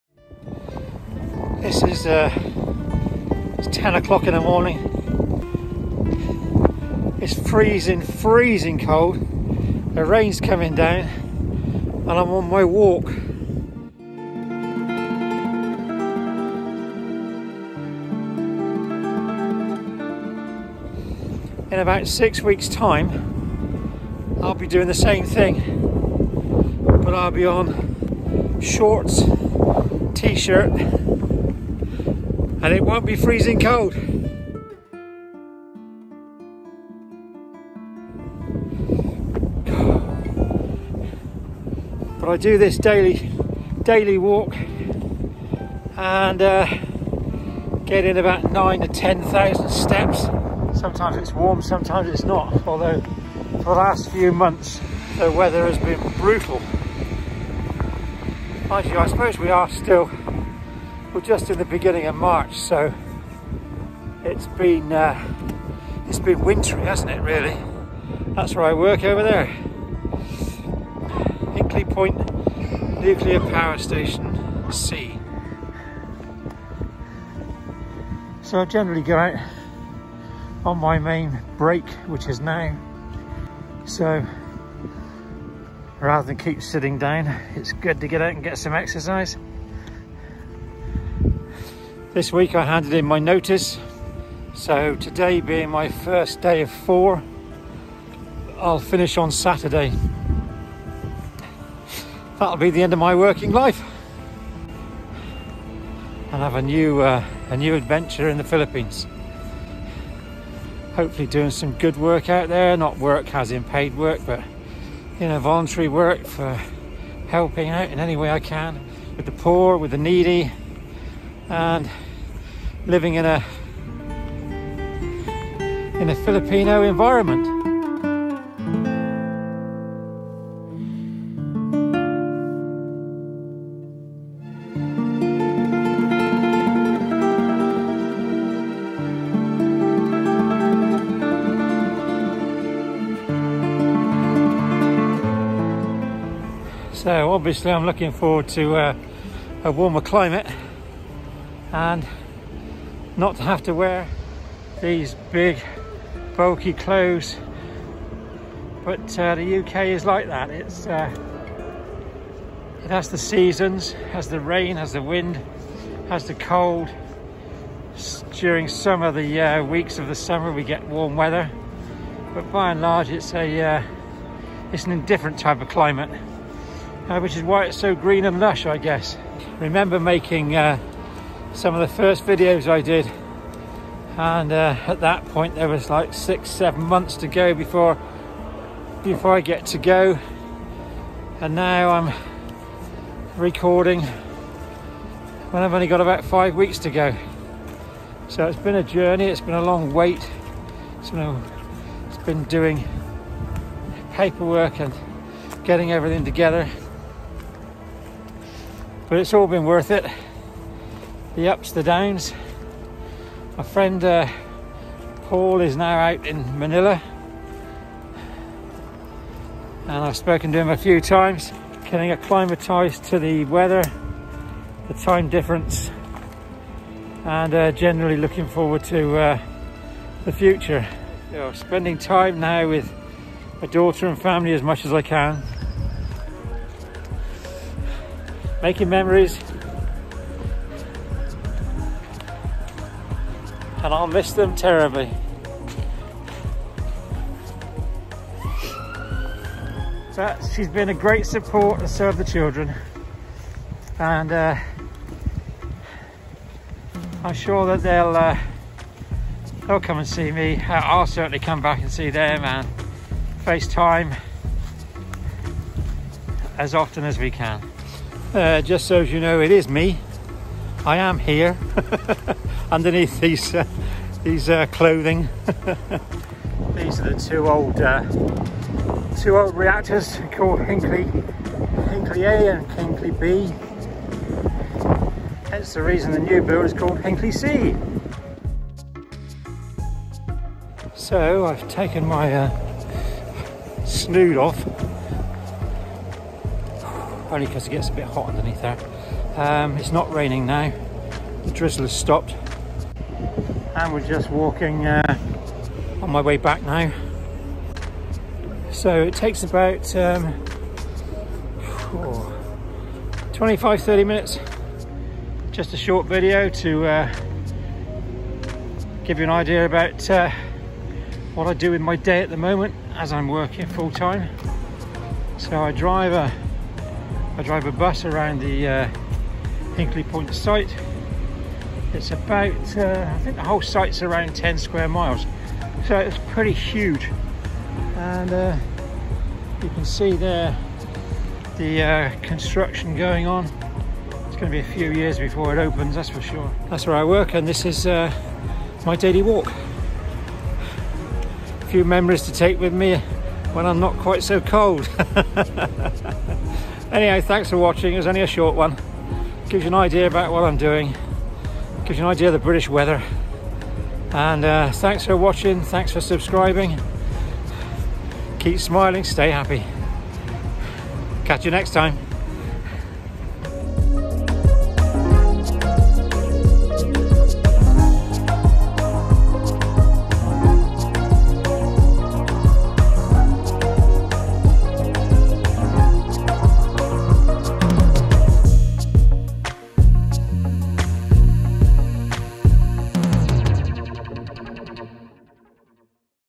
My last 4 days at work - 6 Weeks to go!!!
This is Day One of 4 left until I retire, talking about the next 6 weeks
Thank you for following my journey so far with my life in the UK over the last 6-8 months
Today I gave notice that I will be retiring from work.
I will share my last day at work with you
Keep smiling

I have subsequently returned to the UK ffrom the Philippines, or family health reasons. I will return in the future, but I don't know when
Thank you for your support and will be vlogging more from UK - October 2022